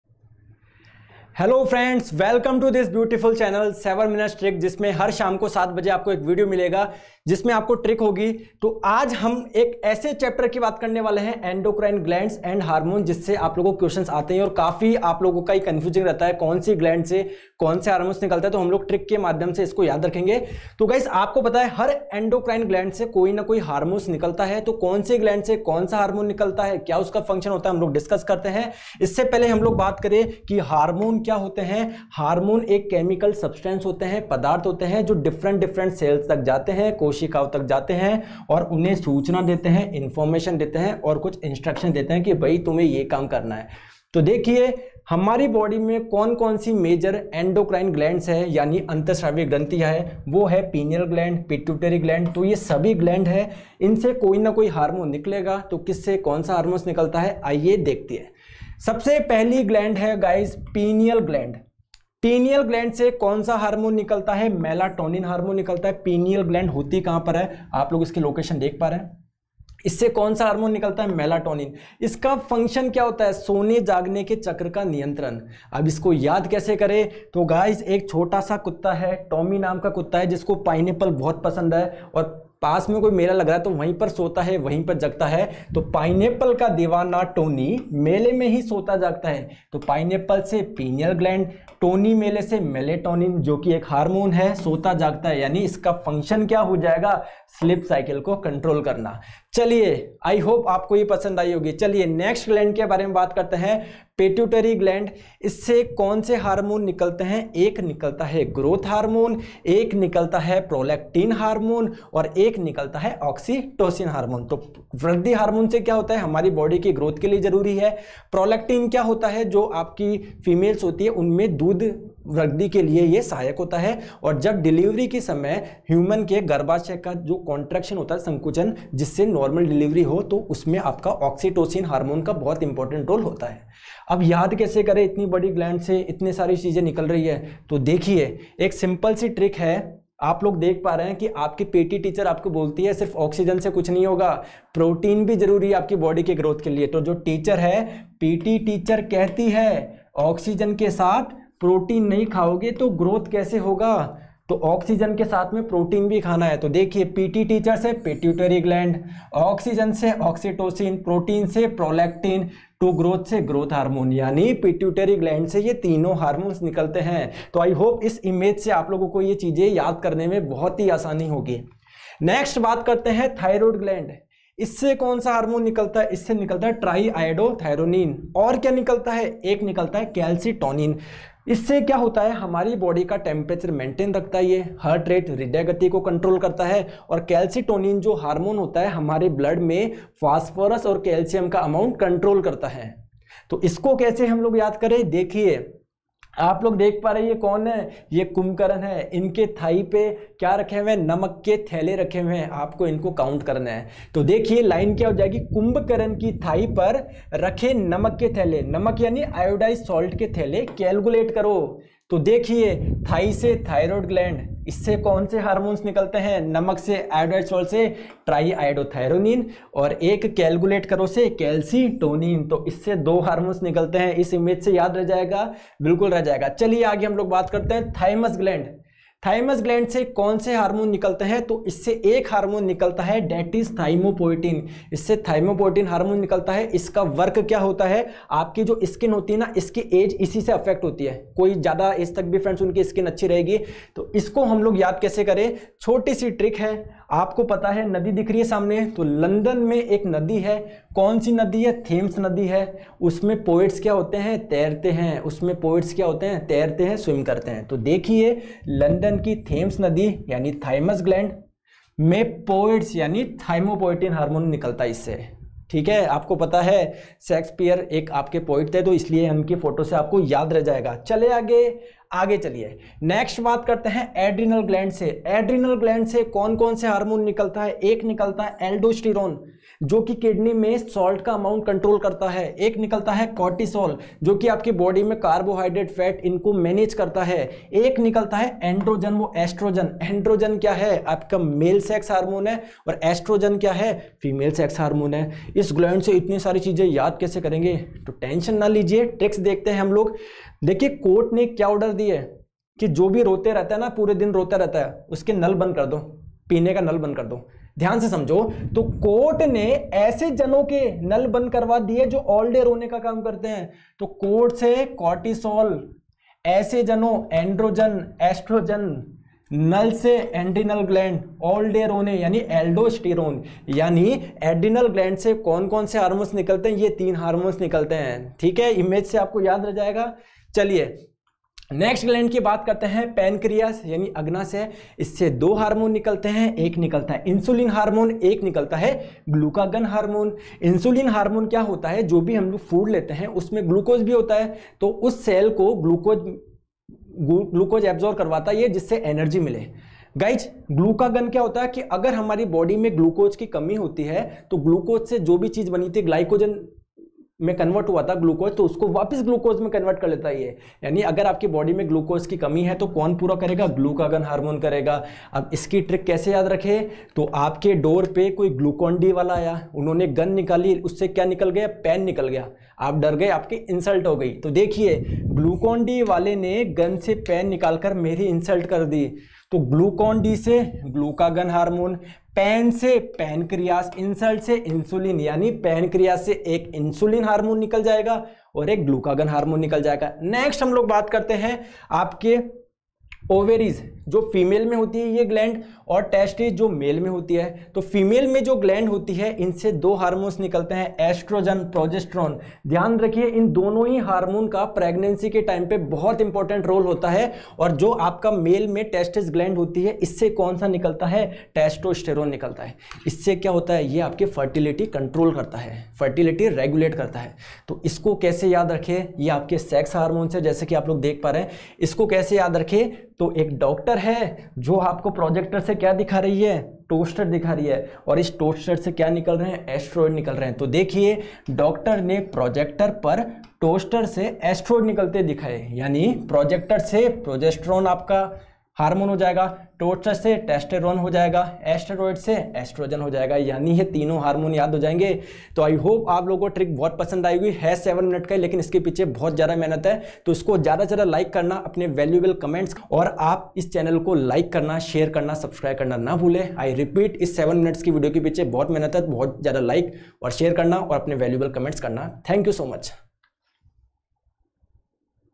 7-Minute GK Tricks | Endocrine Glands and Hormones (ग्रंथियां और हार्मोन) | Science by Neeraj Sir
7-Minute GK Tricks | Endocrine Glands and Hormones (ग्रंथियां और हार्मोन) | Science by Neeraj Sir

Neeraj Sir has discussed Endocrine Glands and their Hormones (अंत:स्त्रावी तंत्र की ग्रंथियां और उनसे ​रक्त में स्त्रावित होने वाले हार्मोन), which is a very important topic in Science (Biology and Human Physiology). Sir Covered Seven Important and Major Human Body Glands of Endocrine System and Hormones secreted by those.

With visually rich presentation, short and quick memorization tricks (storytelling and association) and memory hacks, it will the best free video on General Science.

Glands and Hormones Covered:
0:58 PINEAL GLAND / गावदुम / पीनियल ग्रंथि
1:42 PITUITARY GLAND / पीयूष ग्रंथि
2:50 THYROID GLAND / अवटु /  थायराइड ग्रंथि
3:46 THYMUS  GLAND / थायमस ग्रंथि
4:32 ADRENAL GLANDS / आधिवृक्क / एड्रि‍नल ग्रंथि
5:32 PANCREAS / अग्न्याशय
6:30 OVARIES (FEMALE) / अंडाशय & TESTES (MALE) / वृषण